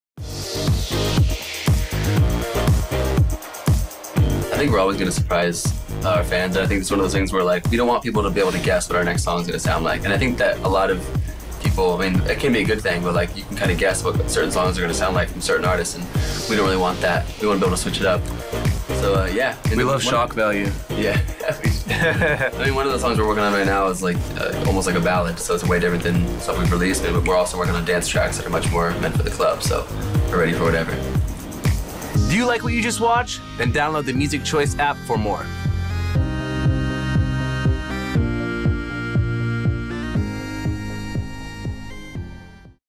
Cheat Codes Says They're All About Surprises
Cheat Codes explains how they never wants to become predictable.